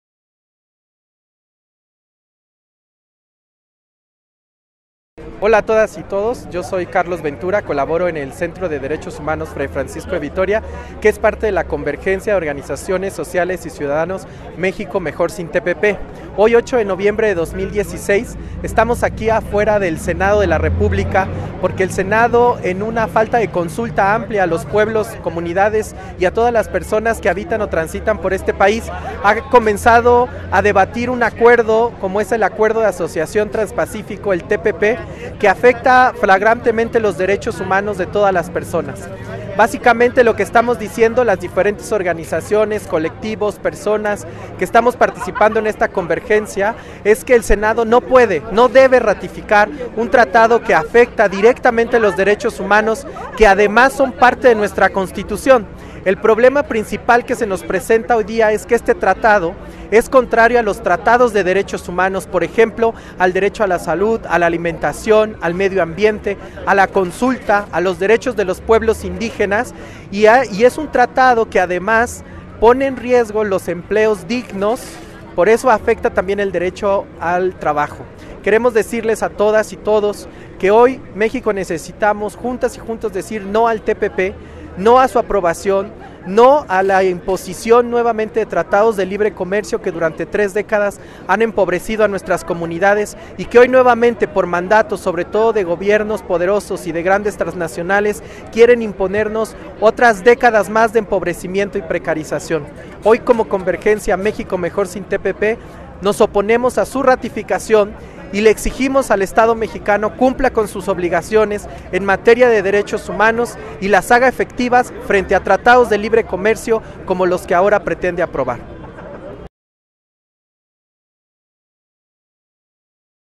Mexico Mejos sin TPP - Carlos Ventura miembro del CDH Fray Francisco de Vitoria.
Mexico Mejos sin TPP - Carlos Ventura miembro del Centro de Derechos Humanos del Centro Fray Francisco de Vitoria.

Exigen a autoridades mexicanas rechazar el Tratado Trans Pacífico TPP.

México, 8 de Noviembre de 2016; Organizaciones agrupadas en el Frente "México mejor sin TPP" se manifestaron frente a la embajada estadounidense y el Senado mexicano para rechazar la intención del gobierno mexicano de aprobar este nuevo tratado comercial de corte neoliberal. Aseguraron que este acuerdo pone en riesgo y vulnerabilidad la salud, la alimentación, el trabajo, el medios ambiente y los derechos humanos de los doce países promoventes. Es de vital importancia rechazar estas políticas internacionales que solo dan mayores ventajas a los capitalistas que hoy depredan el mundo.

Esta campaña es parte de una gran Jornada Continental contra estos acuerdos neoliberales y privatizadores. El TPP esta en revisión durante dos años y de ser aprobado por el mal gobierno significaría la agudización del sistema de explotación. desigualdad y depredación que hemos `padecido en los últimos acuerdos neoliberales como el Tratado de Libre Comecio de América del Norte TLCAN.